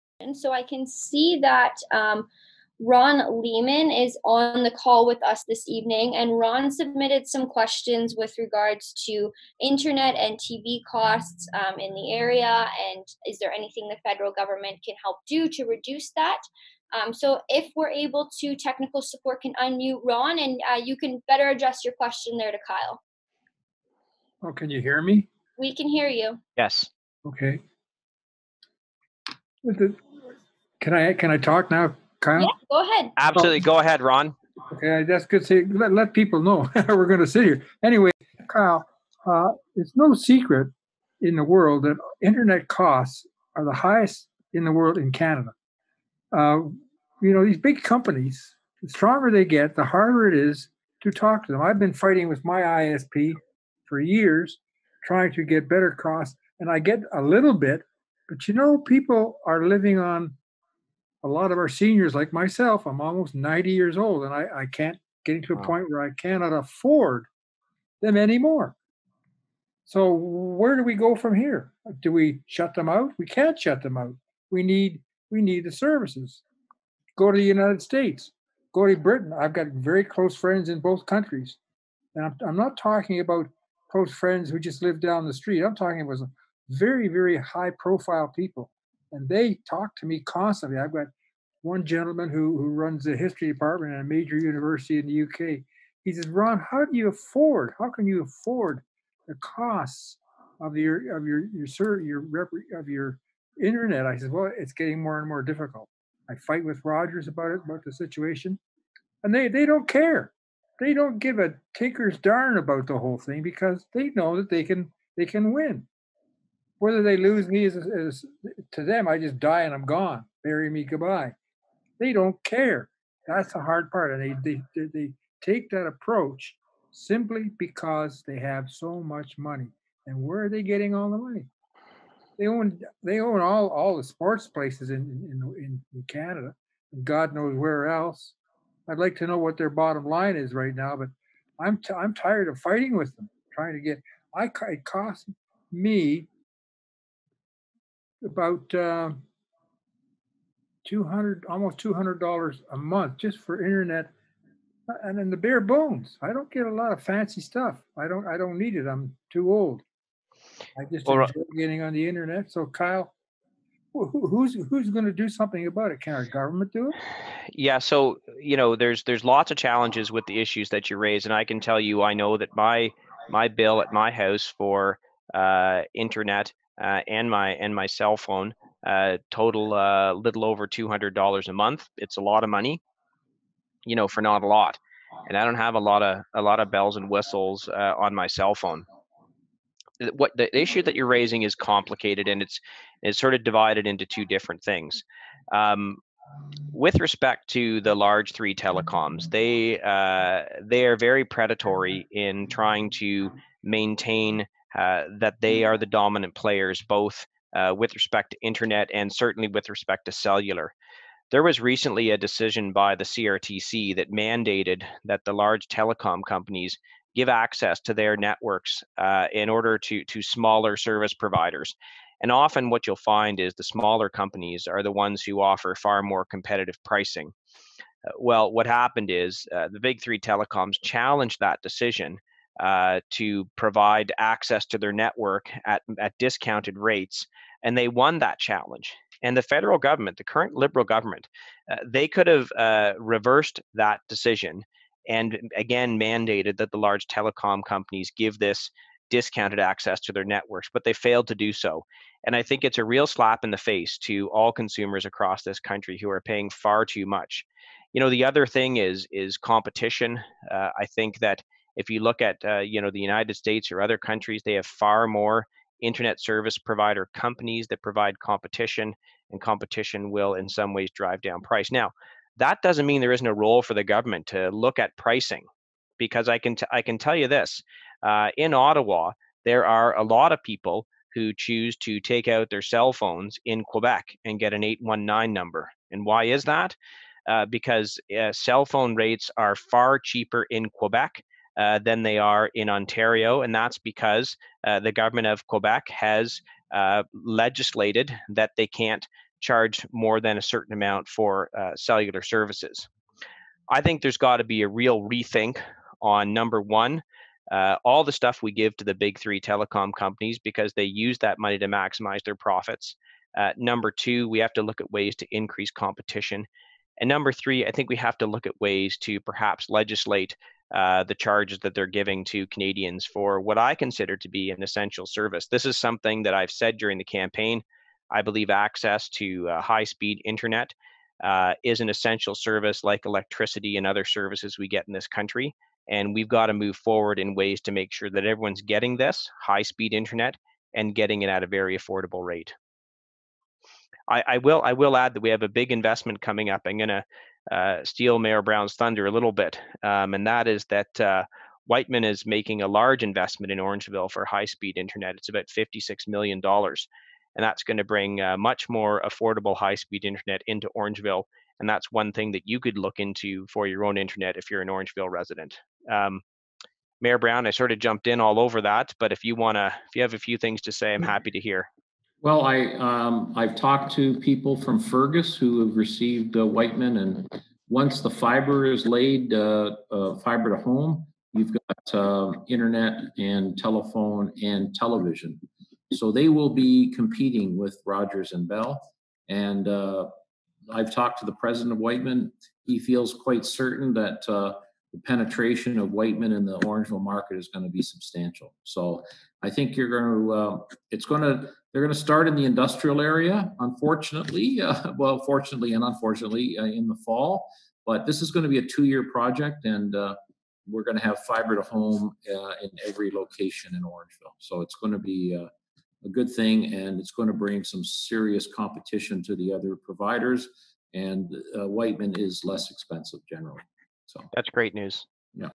Internet Cost | Orangeville Virtual Town Hall Meeting, Sept. 02, 2020
Important discussion on various federal and municipal matters with Orangeville residents and Mayor Sandy Brown. Thank you everyone who registered and participated in my 5th town hall meeting this year.

In this clip, I am answering Ron's question: "Why are the fees for our TV and internet so high in Canada and yet in other countries they are substantially lower with service as good or better than we receive and when is our Federal Government (who know about this problem) going to force suppliers to lower their costs?"